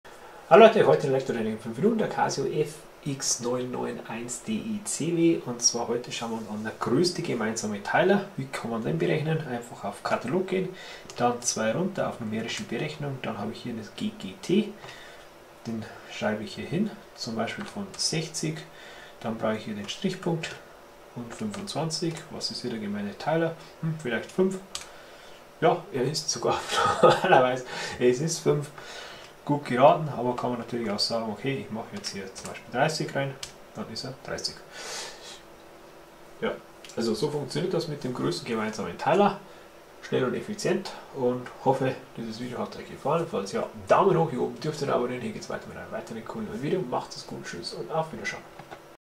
ggt größter gemeinsamer Teiler Casio fx991 DE CW
Heute zeige ich wie ihr mit dem Casio fx991 den größten gemeinsamen Teiler berechnen könnt
Anzeige